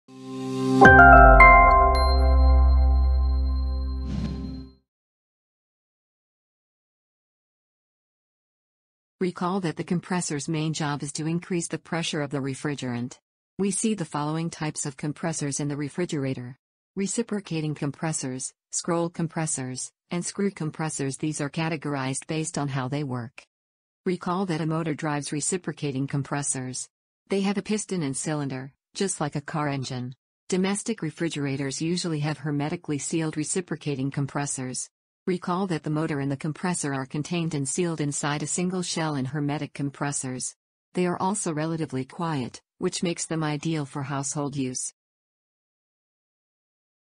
All About Compressor | Working of Domestic Refrigerators Course | SkillCat
Learn about compressor in this chapter of working of domestic refrigerators Course.